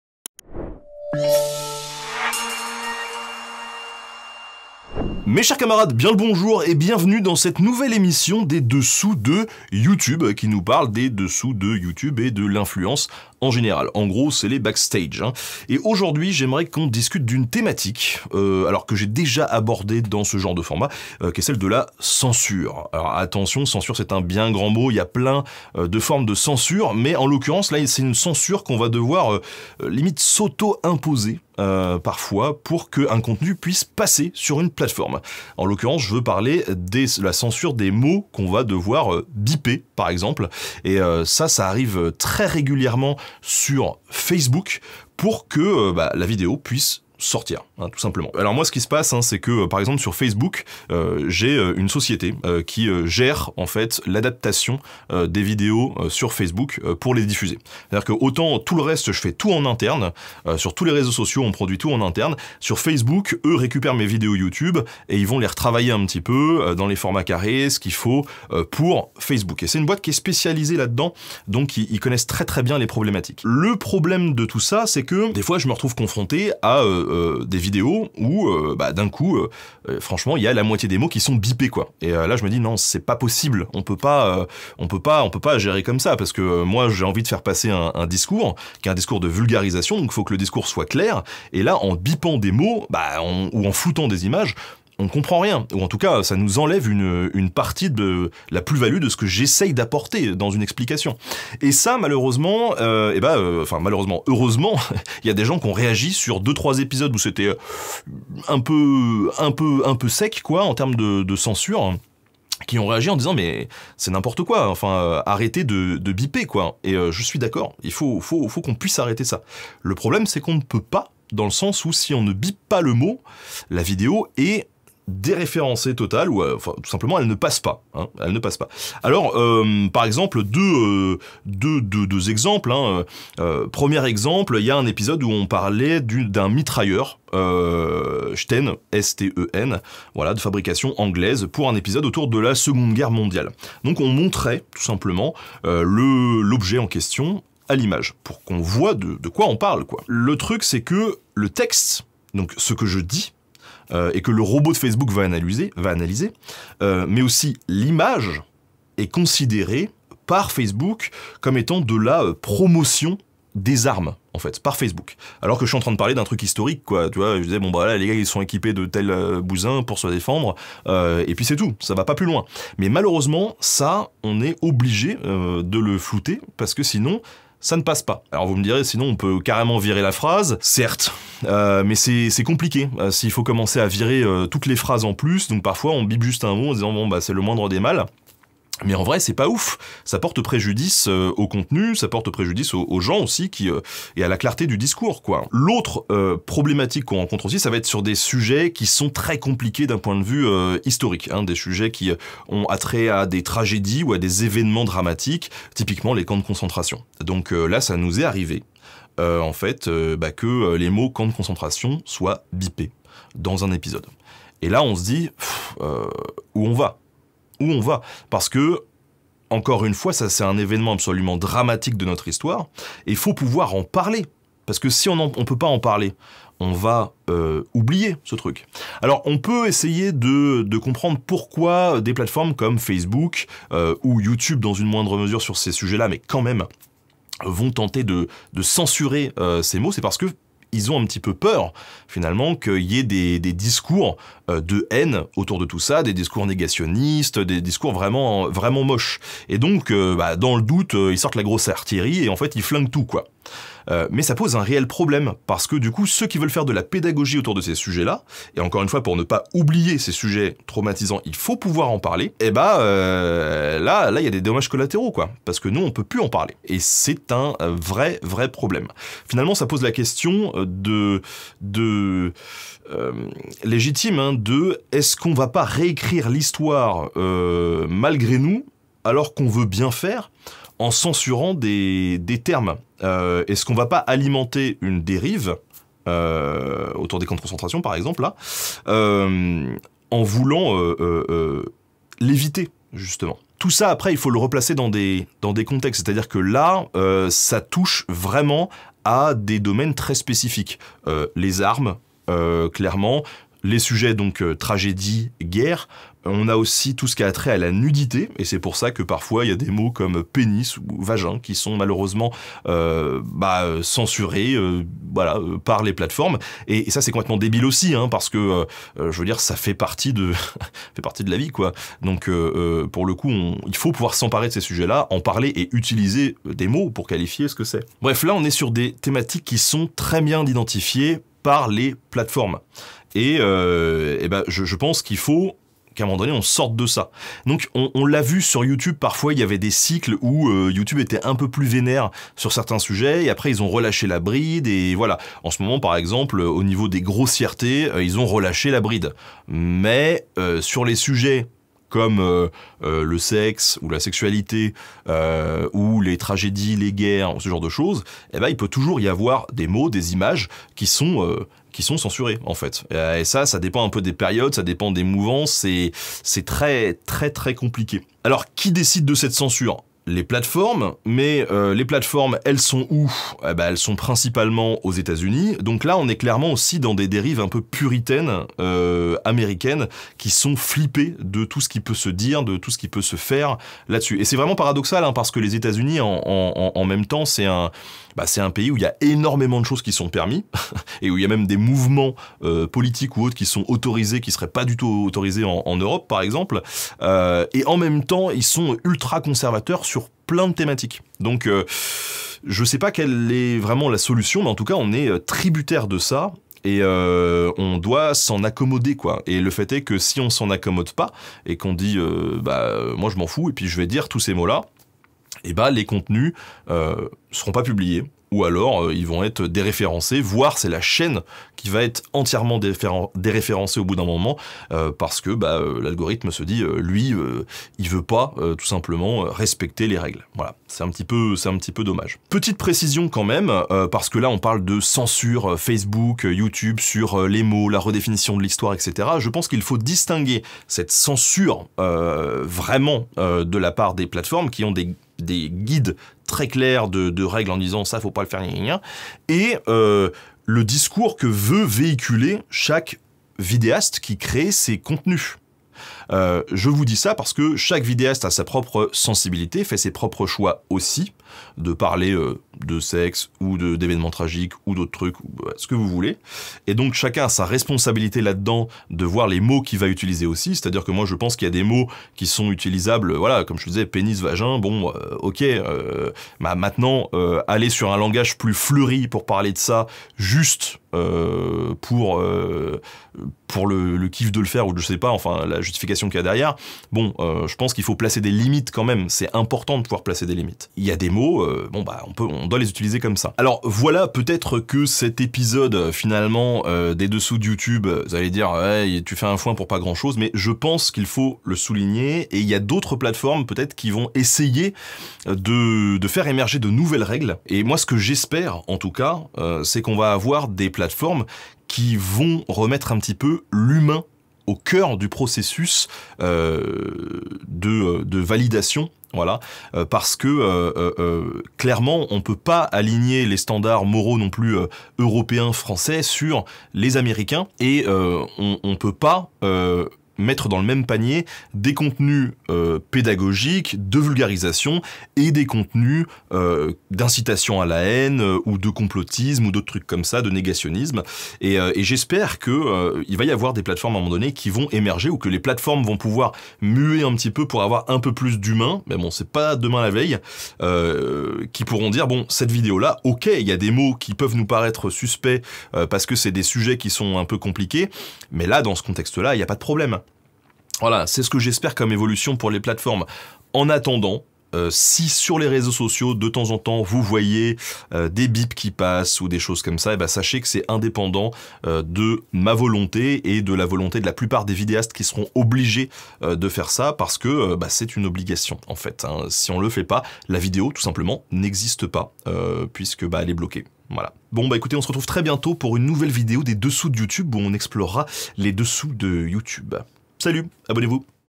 Encore une censure des mots chez Facebook ?
Bienvenue dans cette nouvelle émission des Dessous de YouTube ! Aujourd'hui j'aimerais discuter d'une thématique que j'ai déjà abordée dans ce genre de format, qui est celle de la censure. Alors attention "censure" est un bien grand mot, il y en a plein de formes différentes, mais en l'occurrence ici c'est une censure qu'on va devoir limite s'auto imposer parfois pour qu'un contenu puisse passer sur une plateforme. En l'occurrence je veux parler de la censure des mots qu'on va devoir biper par exemple, et ça, ça arrive très régulièrement sur Facebook, pour que la vidéo puisse sortir tout simplement !

🔸Découvrez ma boutique en ligne🔸